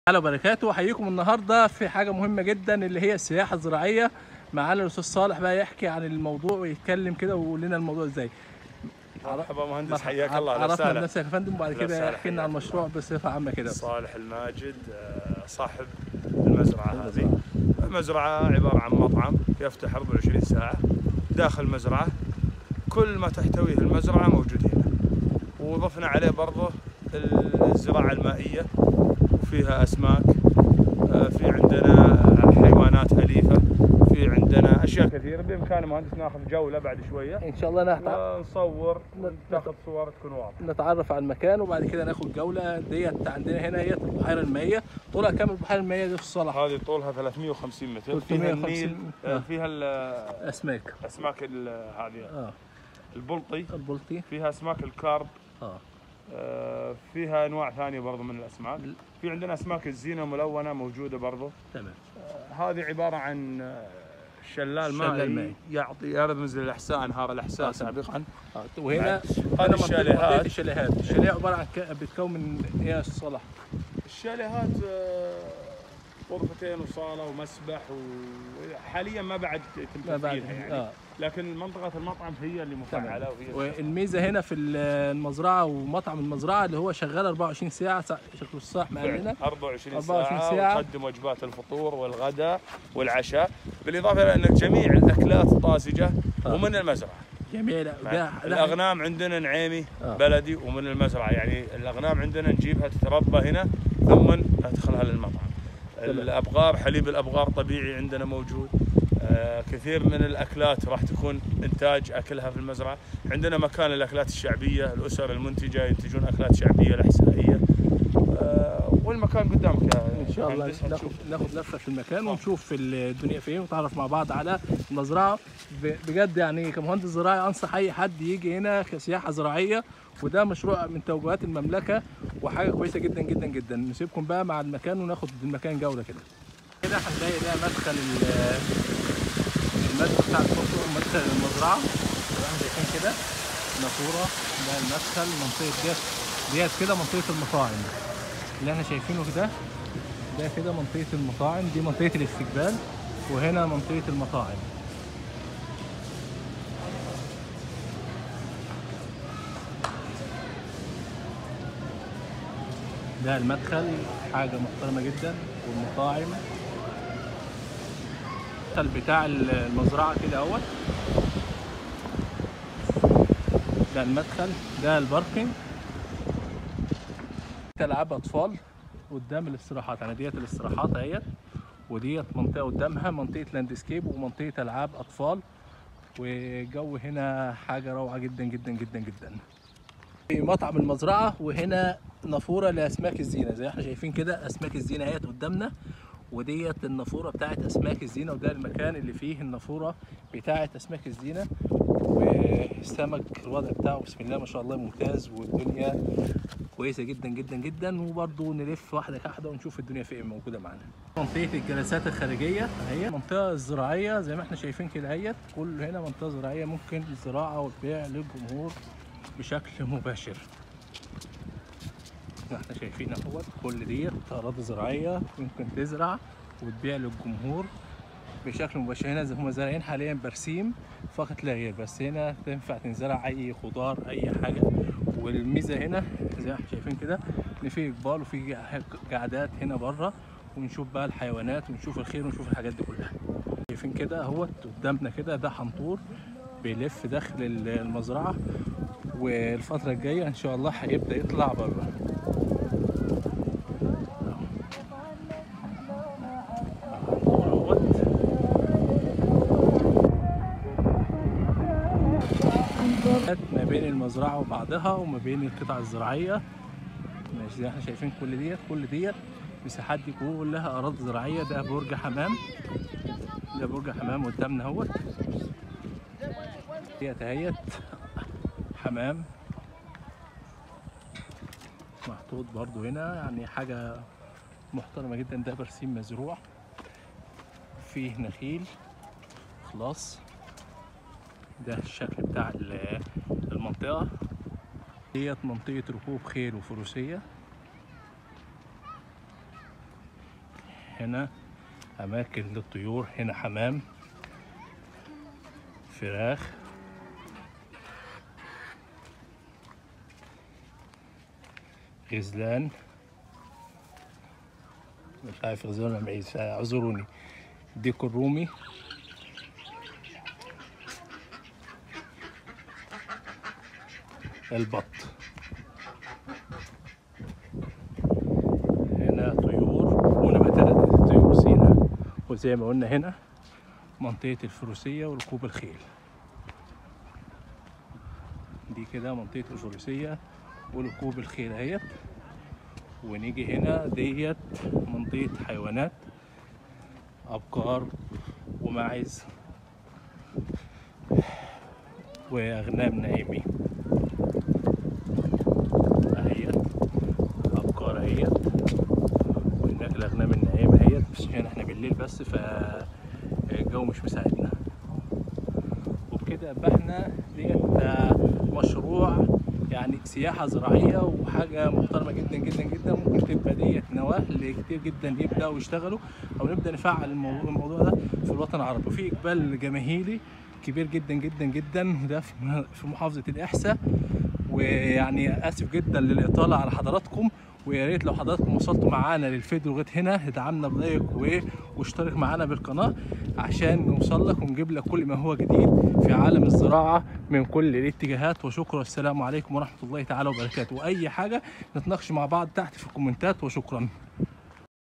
السياحة الزراعيه في الوطن العربي
السياحة الزراعية هي احدي عوامل النهوض بالمجال الزراعي
التعريف بالسياحة الزراعية
التعرف علي مشروع السياحة الزراعية في الاحساء المملكة العربية السعودية
جوله في المشروع والتعرف علي مكونات المشروع وهي التالي
مدخل خاص
منطقة مطاعم
منطقة استراحات
منطقة جلسات خارجية
منطقه العاب أطفال
مسجد
نهر صناعي بطول ٣٥٠ متر
الحنطور
المنطقة الزراعية
برج الحمام
منطقه الفروسية وركوب الخيل
حظائر الماشية
مهندس زراعي محمد سالم
مهندس محمد سالم
قناة النخلة
قناة النخلة الزراعية
مزرعة الياسمين العضوية
The Palm